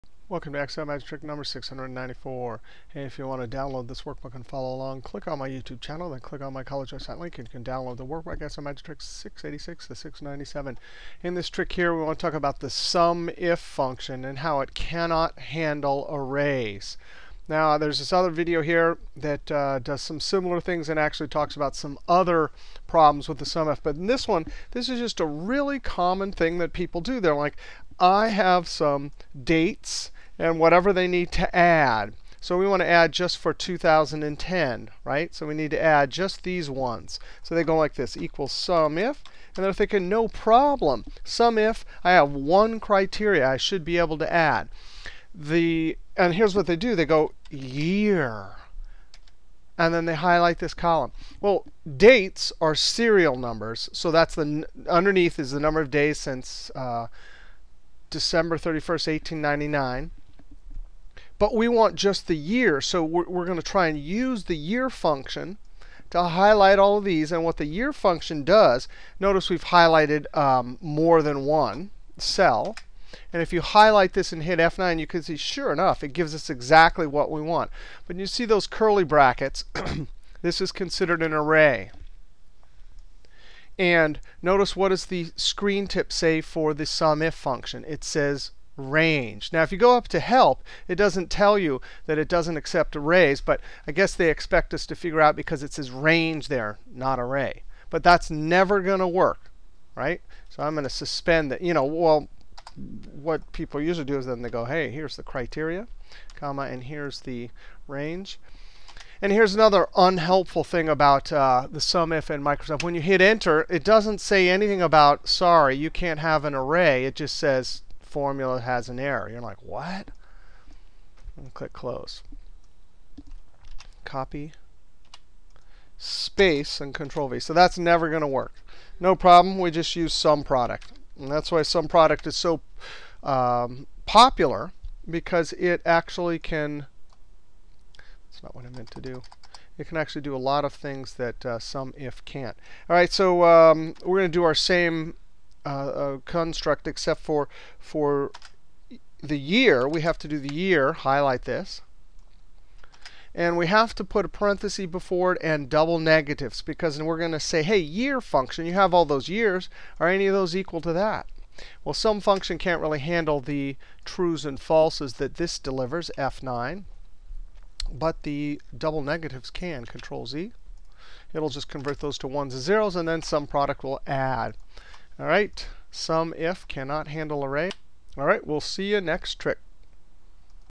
Excel Magic Trick 694: SUMIF Can't Handle Arrays. SUMIF(YEAR(A2:A10),2010,B2:B10) Won't Work
Learn about how the SUMIF function (and COUNTIF, etc.) cannot accept arrays. If you need to sum by year from transactional data the SUMIF and YEAR functions cannot be used together. Instead we can use the SUMPRODUCT function. Adding with 1 one criteria condition.